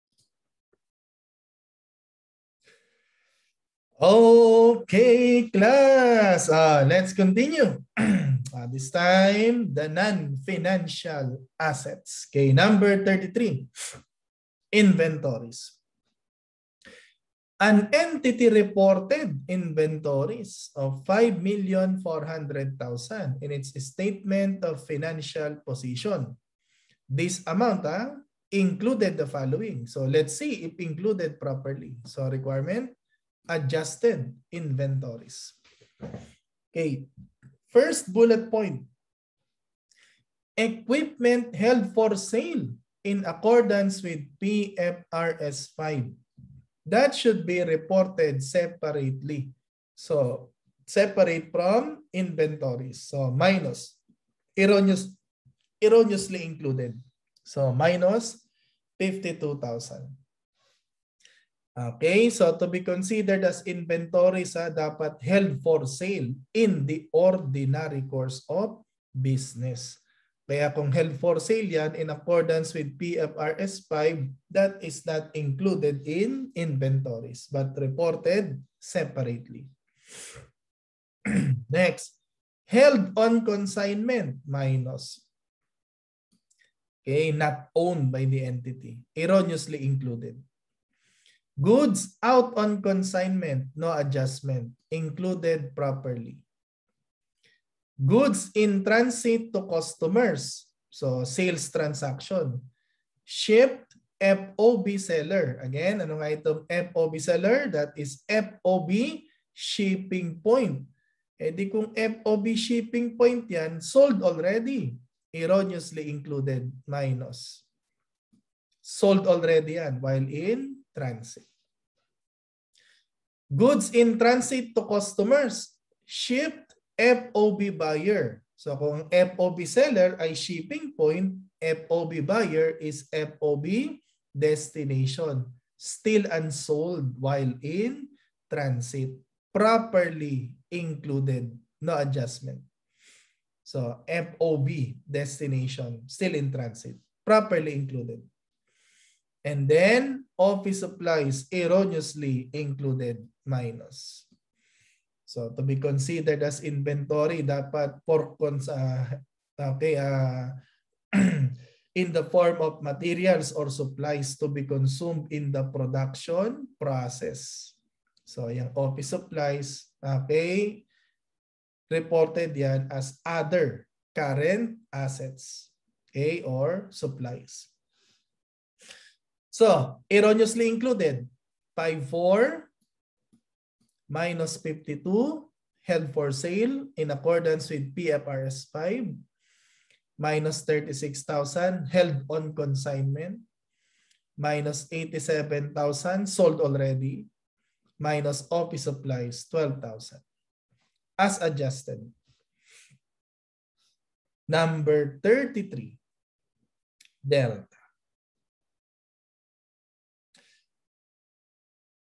FAR Problem of the Day (June 11, 2022) - Items Considered as Inventories
Solution to ‘Problem of the Day’ posted June 11, 2022 on Rey Ocampo (FAR and AUD reviewer, Team PRTC) FB Group